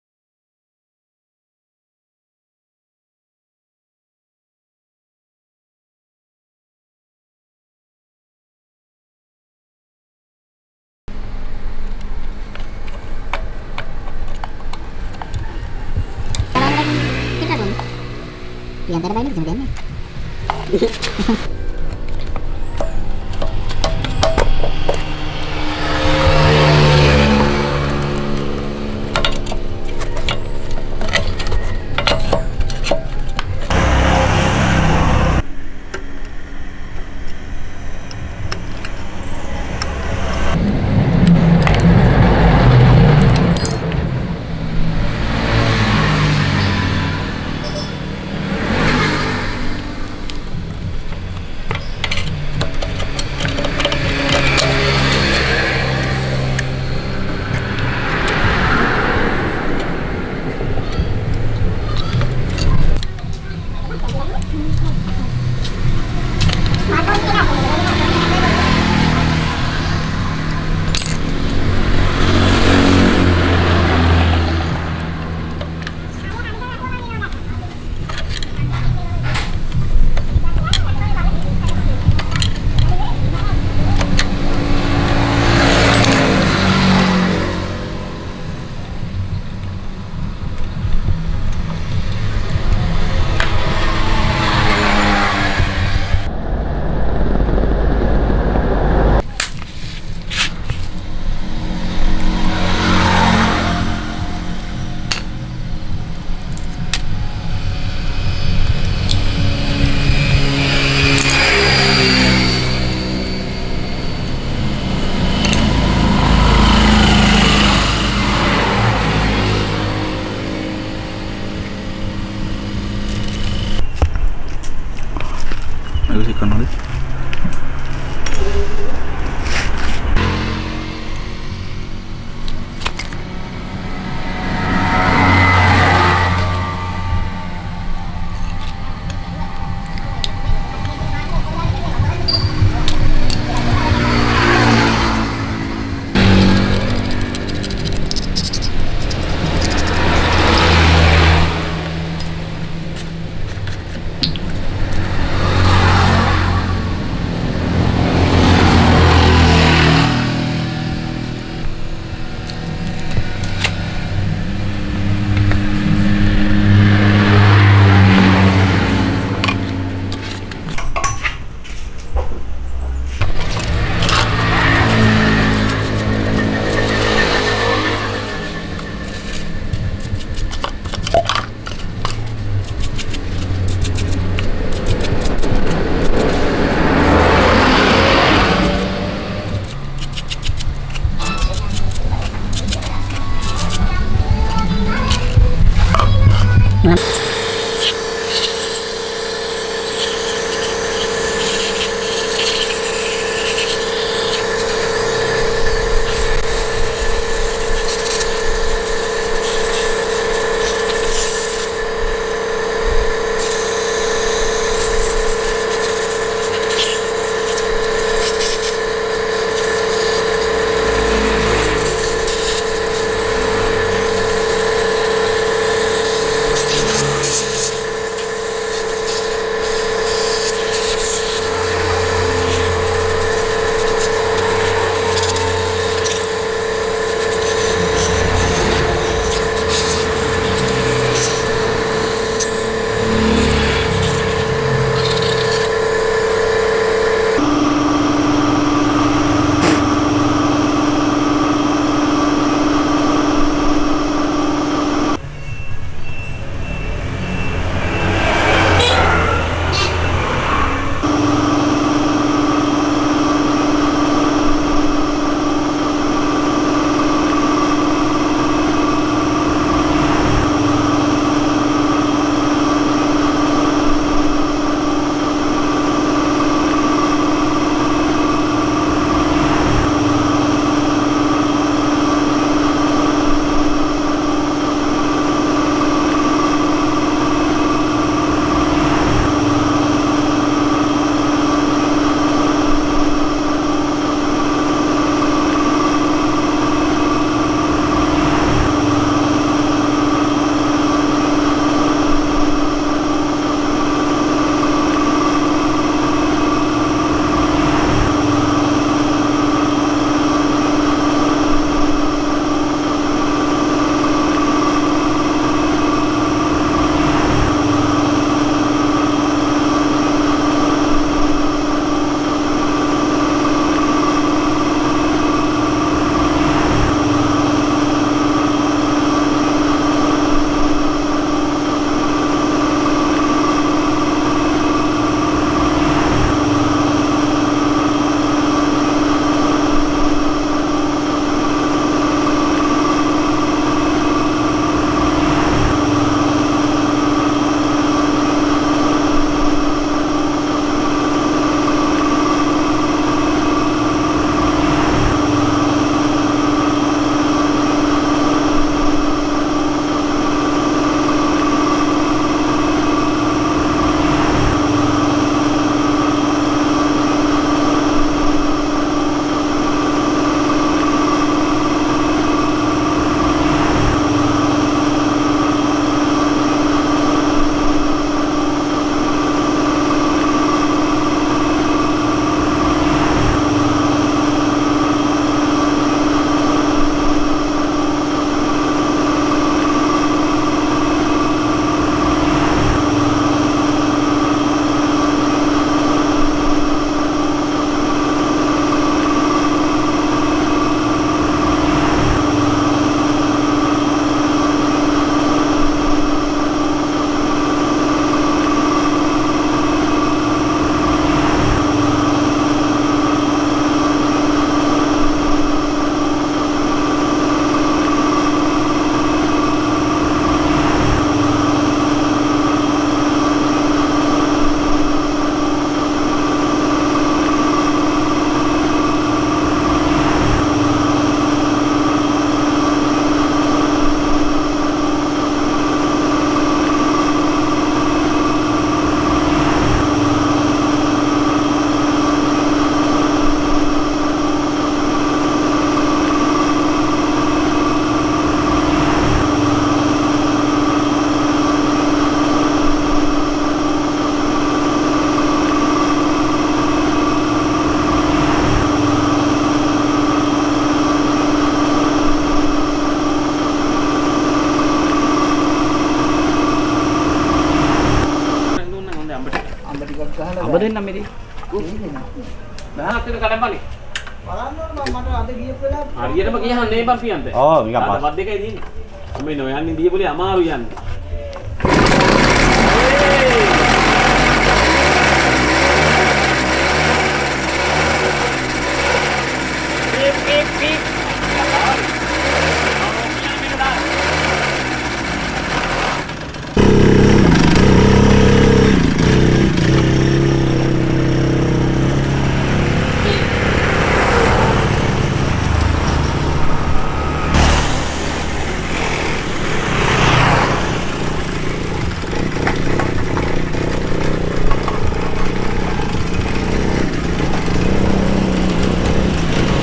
TVS Champ Bike Carburetor Repair | Very old TVS Champ engine carburetor cleaned and fixed.
In this video we are repairing the carburetor on a TVS champ motor bike. bike has a starting issue and the engine does not start. the carburetor is not replaced but taken apart, cleaned and fixed successfully to start the bike.